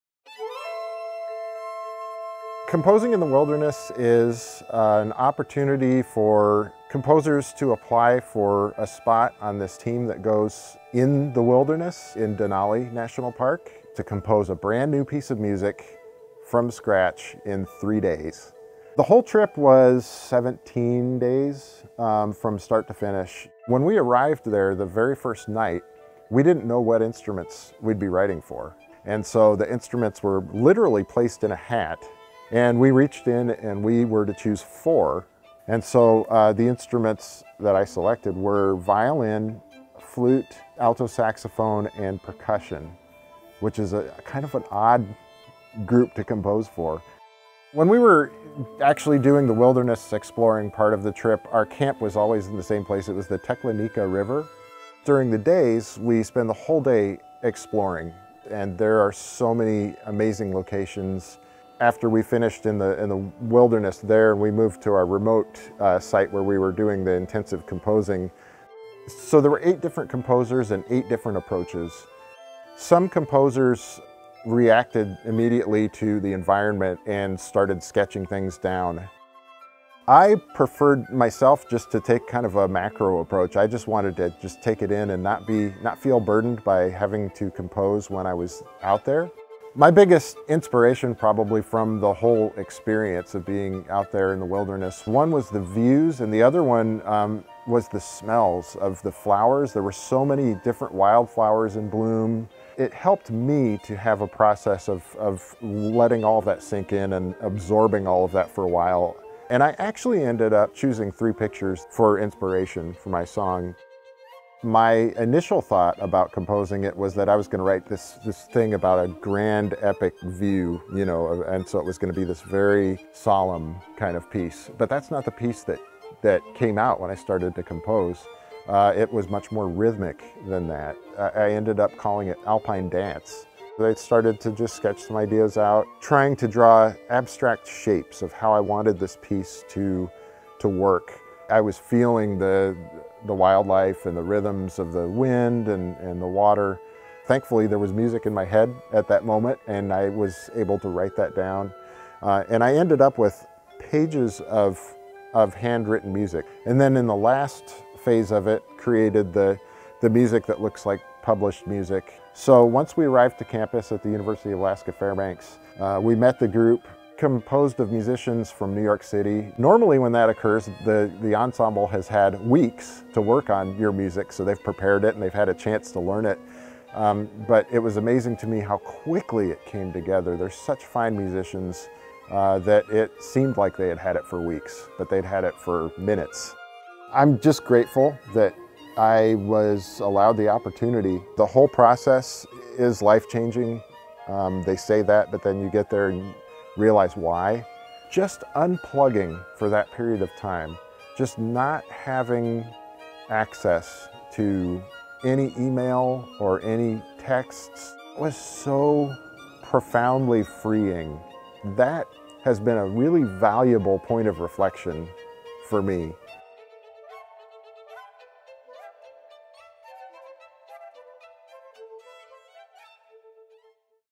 Composing in the Wilderness with Dr. Christopher Schmitz
This summer, Mercer School of Music professor Dr. Christopher Schmitz spent two weeks in the Alaskan wilderness and created a piece of original music about his experience as a participant in the Composing in the Wilderness program.